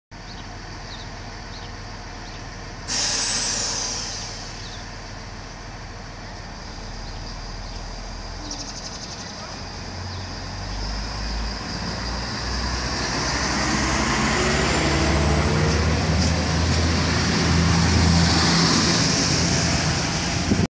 Euregiobahn in Eschweiler-West
EW-West ist ein Bedarfshalt zwischen Stolberg und dem Talbf und bindet unter anderem die Euregiobahn an die Häuser am Hbf und den Westen der Stadt an.